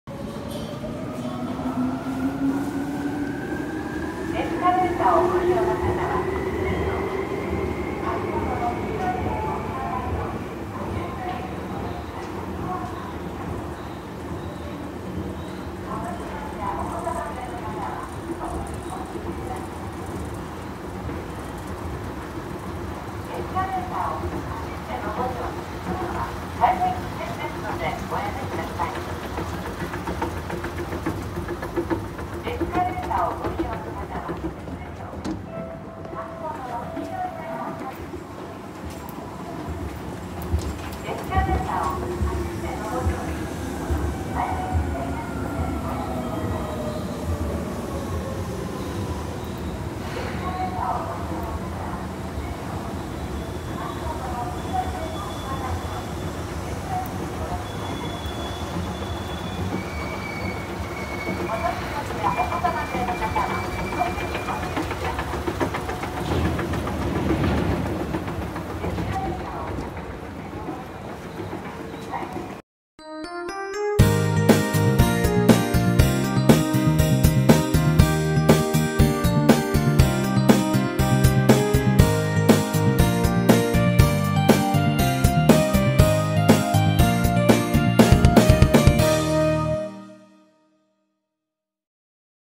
東大和市駅 エスカレーター【西武拝島線】
FUJITEC

［チャプター］
0:00 改札階→1番ホーム
0:38 改札階→2番ホーム
1:13 ご視聴ありがとうございました